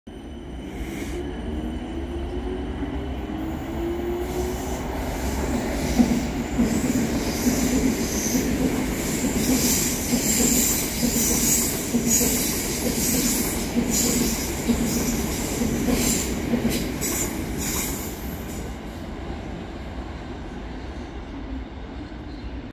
Class 450 speeds through Vauxhall
Watch this class 450 as it speeds through Vauxhall.

Filmed, Uploaded, and Thumbnail by - Ben Tuppenney